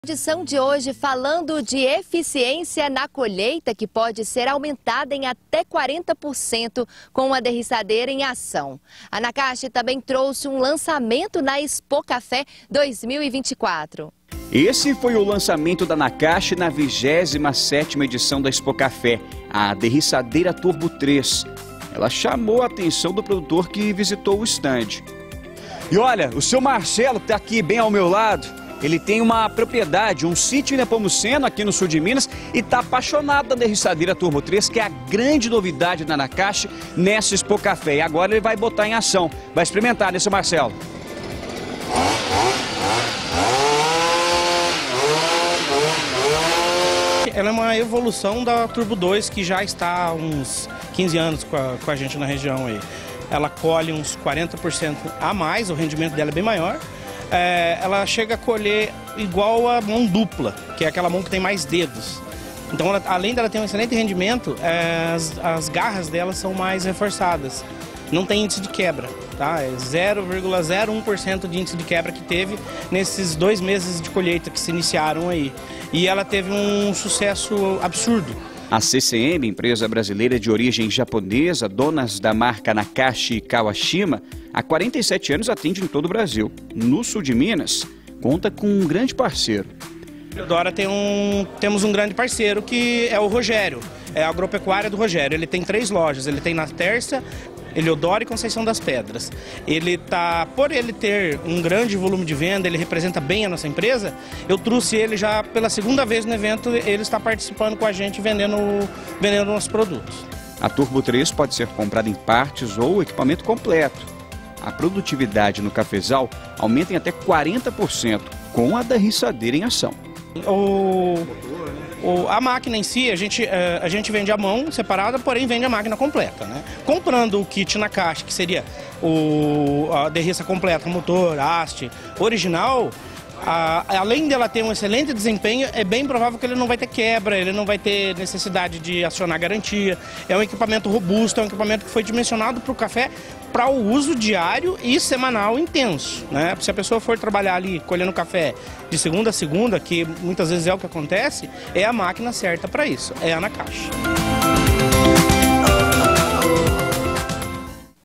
Entrevista na Expocafé 2024 sobre o Kit Derriçador Turbo 3 Nakashi
Assista a reportagem feita em nosso estande, sobre o novo Kit Derriçador Turbo 3.

Conheça o canal da TV Alterosa:
   /tvalterosasuldemg